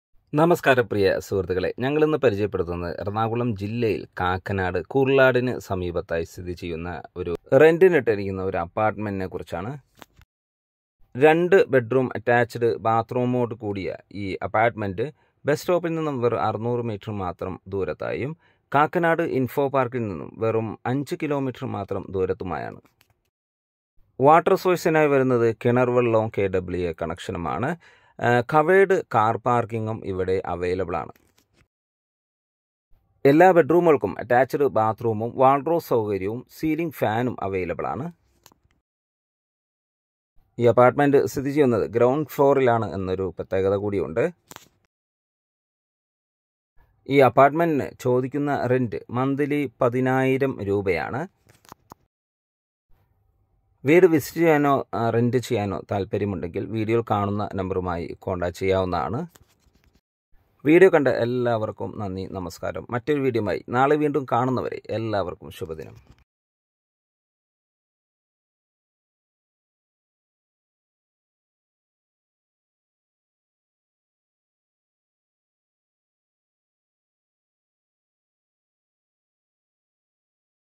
ID. 555/ 2 BHK Apartment for Rent @Eranakulam near Kakkanad
Location - Eranakulam,  near Kakkanad
kurlad

2 bhk house for rent
Rent - ₹10000 Per Month

For all budget house property videos